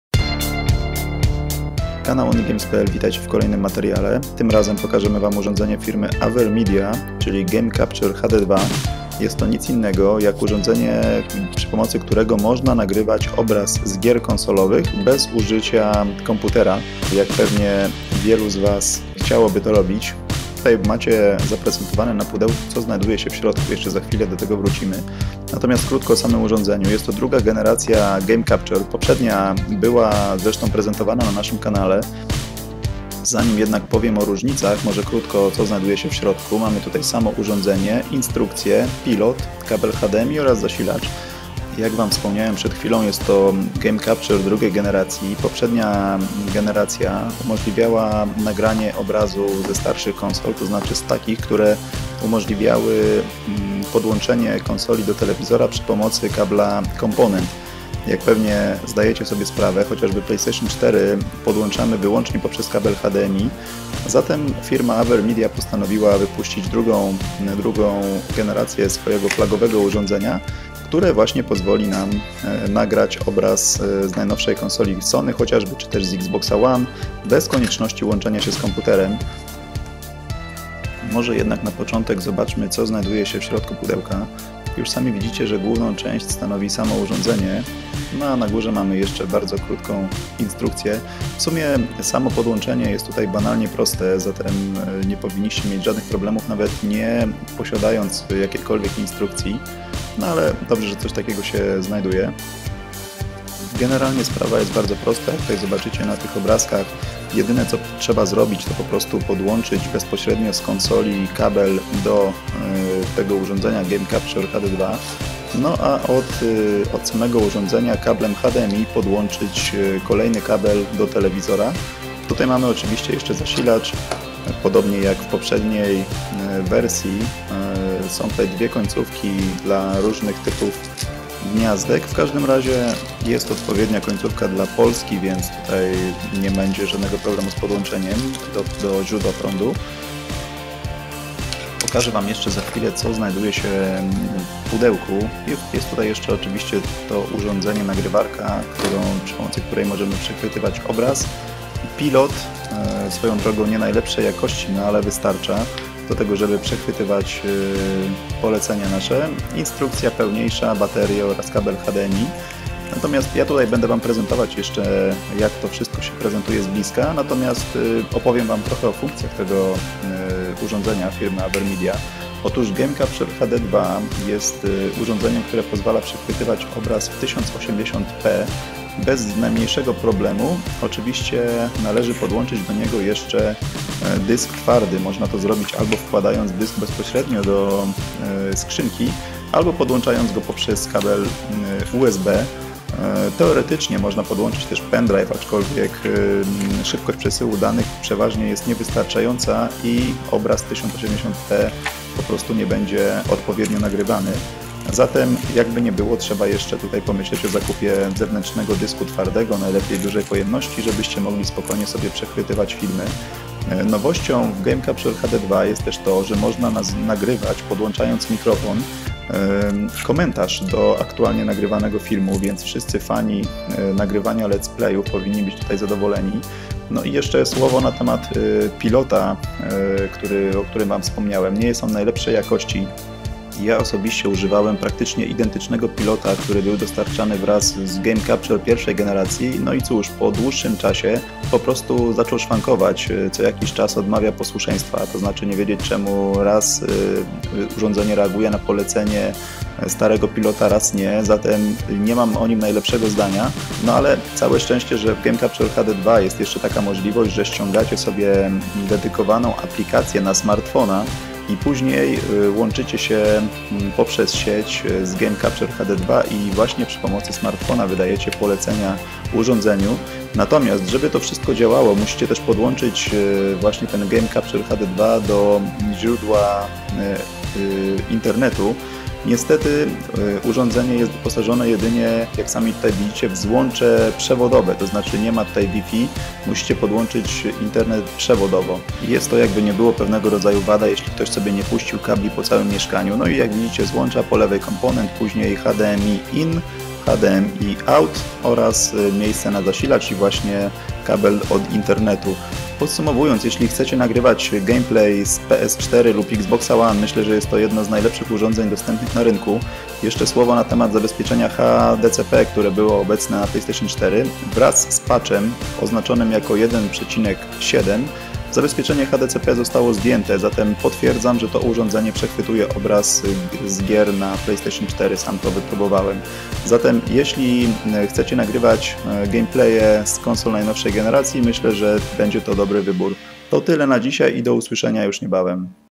AVERMEDIA GAME CAPTURE HD 2 - unboxing & test (jak nagrać obraz z PS4 i Xboxa One bez używania PC)
Przed Wami unboxing oraz mini-test Game Capture HD II firmy Avermedia. Jest to sprzęt, który pozwala nagrywać filmiki z PS4, Xboxa One, a także starszych konsol obsługujących component (PS3, PS2, Xbox 360, Wii, Wii U) bez używania do tego celu komputera. Wystarczy jedynie podłączyć przenośny dysk twardy, wcisnąć "record" i gotowe - filmik jest zapisywany w formacie MP4 na dysku.